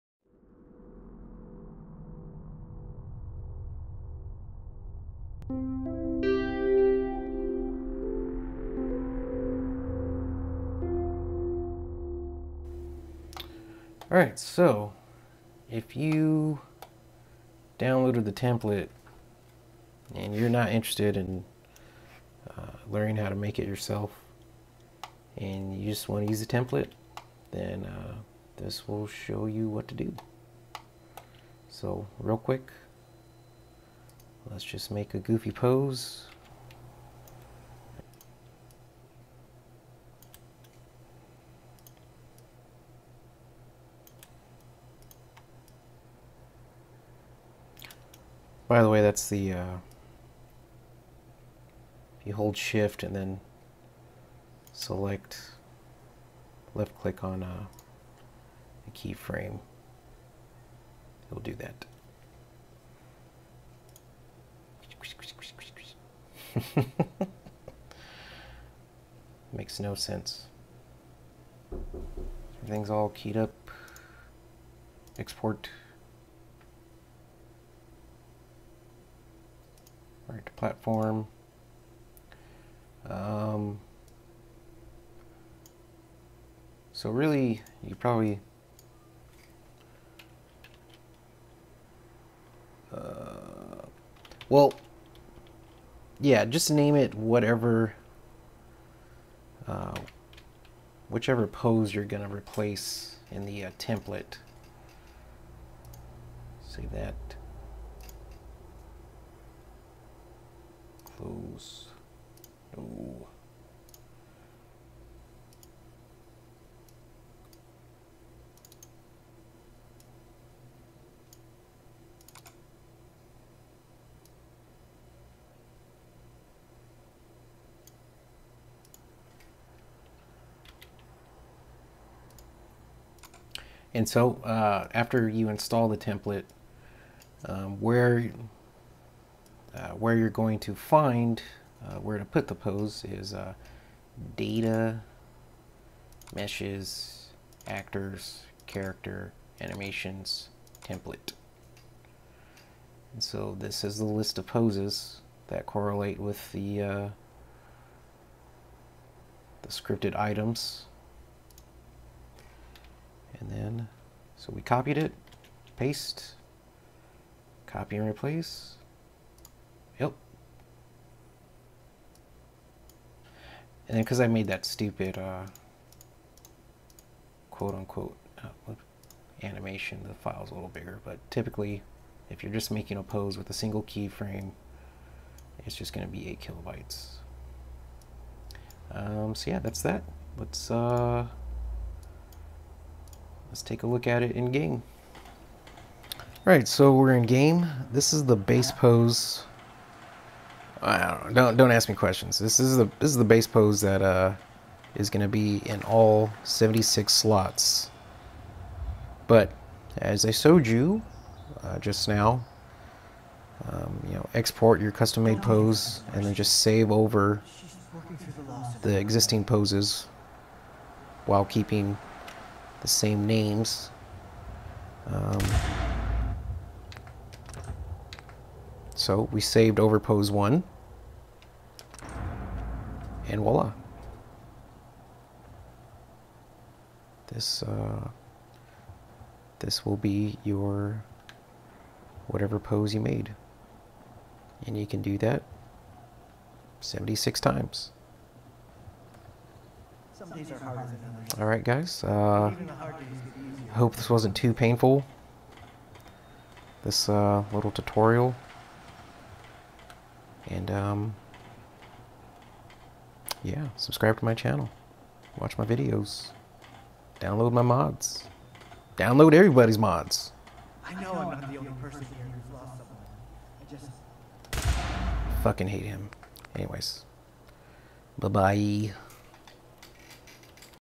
Pose Template Only
This is a video for users who just want to use the pose ring and gun template to quickly add custom poses.  If that describes you, you do not need to watch the other videos.

To use this template you'll need F4biped animation rig and 3ds max 2015 with the Havok Content Tools plugin to make poses.